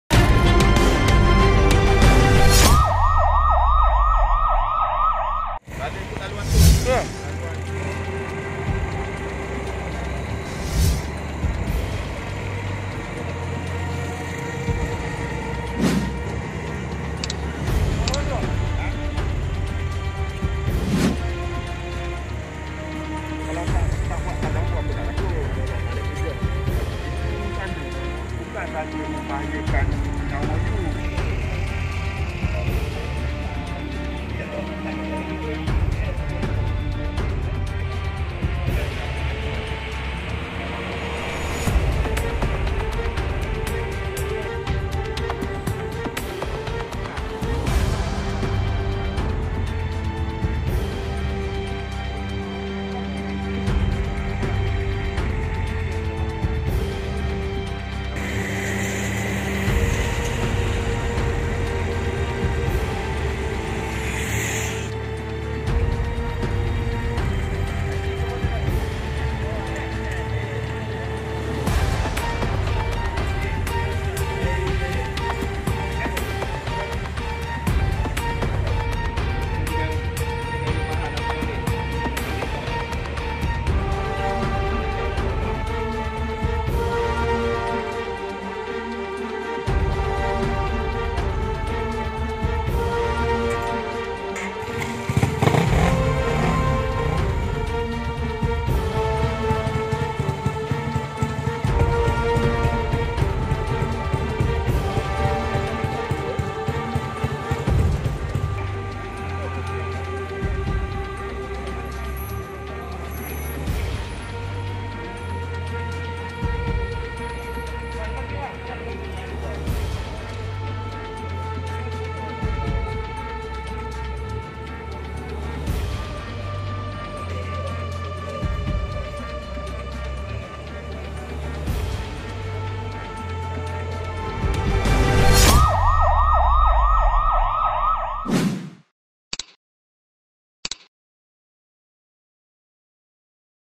MKL Crimedesk | Penunggang Lari SJR Ops Ekzos
elak lalu sekatan jalan raya... penunggang motosikal sanggup pecut laju memasuki lorong kenderaan biasa.

itu antara situasi di operasi bersepadu jabatan siasatan dan penguatkuasaan trafik jspt bukit aman di plaza tol sungai ramal kajang siang tadi.

operasi bertujuan mempergiatkan ops khas bunyi bising dan ubah suai ekzos itu diketuai inspektor penguatkuasa lebuhraya zon tengah b2 jspt bukit aman inspektor johanis wahab.

hasil beberapa jam beroperasi, sebanyak 92 saman dikeluarkan polis bagi pelbagai kesalahan.

turut berada dilokasi penguatkuasaan, jabatan pengangkutan jalan joj, jabatan alam sekitar jas dan agensi anti dadah kebangsaan aadk.

10 motosikal disita kerana mengubah suai ekzos dan tiad lesen memandu.

jas juga mengeluarkan notis kepada 4 kenderaan bagi kesalahan bunyi manakala aadk mengesan 11 pengguna yang positif saringan awal dadah,

menurut pegawai jspt ops khas ini ditujukan khas kepad kenderaan yang ubahsuai ekzos yang mengeluarkan bunyi bising dengan mengambil tindkan kompaun atau sita.

pemeriksaan fizikal dan keselamatan kenderaan lain jug dilakukan jika
ada kesalahan lalulintas .

turut diperiksa rekod kenderaan curi dan  semakan ke atas individu yang yang mempunyai rekod lampau yang berkaitan jenayah dadah.

pemeriksaan juga dibuat ke atas individu yang mempunyai waran tangkap yang aktif.